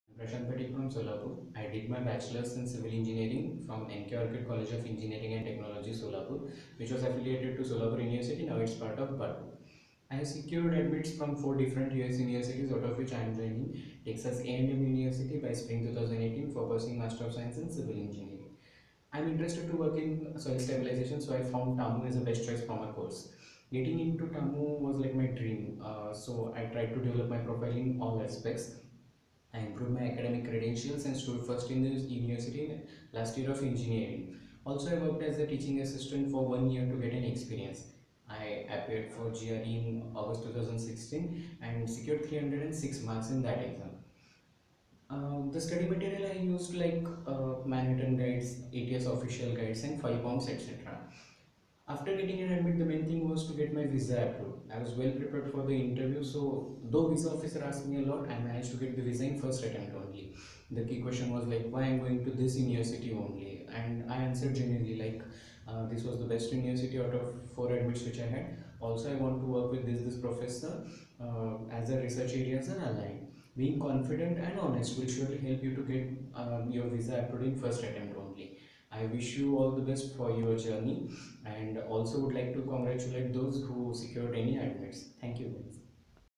Why study at Texas A&M University (TAMU) - MS in Civil Engineering | Study in US
Prashant from Solapur, Maharashtra shares his application experience for MS in US. He will be joining the Texas A&M University to pursue a Masters in Civil Engineering in the Spring 2018 term.

Connect with Prashant and other seniors from TAMU on Yocket - h